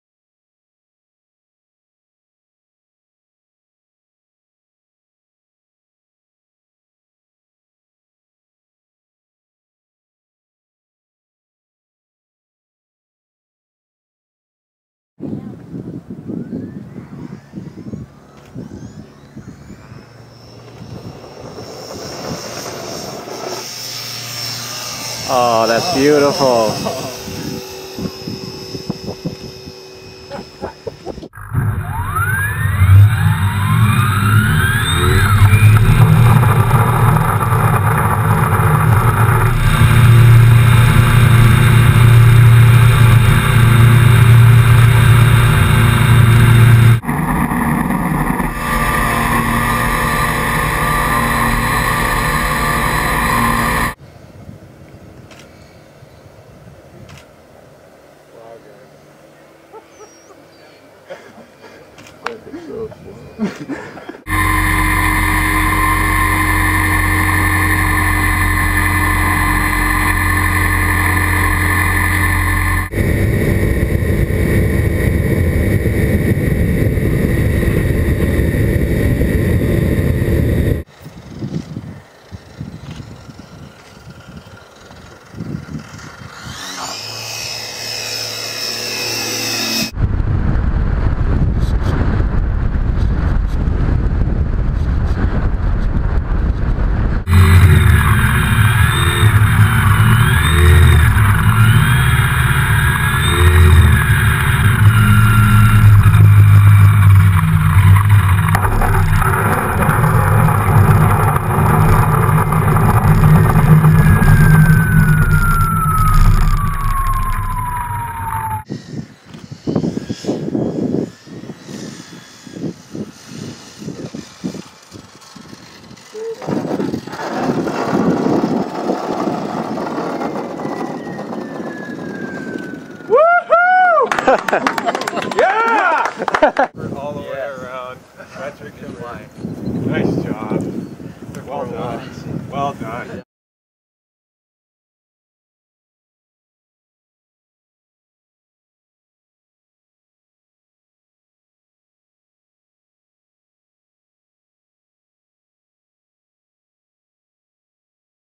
Liquid Hydrogen Fueled Aircraft: Genii UAS First Flight
First flight of the Genii Unmanned Aerial System built by students at Washington State University. This project was funded on June 30, 2012 and completed the first flight on May 18, 2013. The long term goal is to fly with cryogenic liquid hydrogen fuel supplying a proton exchange fuel cell and electric motor (the test flight was powered by batteries).

Flight Time: 16 min
Takeoff weight: 19 kg
Maximum electrical power: 1500 W
Takeoff distance: 20 m
Landing distance: 30 m
Maximum ground speed: 23 m/s, 45 knott, 51 mph
Maximum altitude: 176 m AGL, 580 ft AGL
Location: Lewis-Clark Model RC Club at Mann Lake, Lewiston, Idaho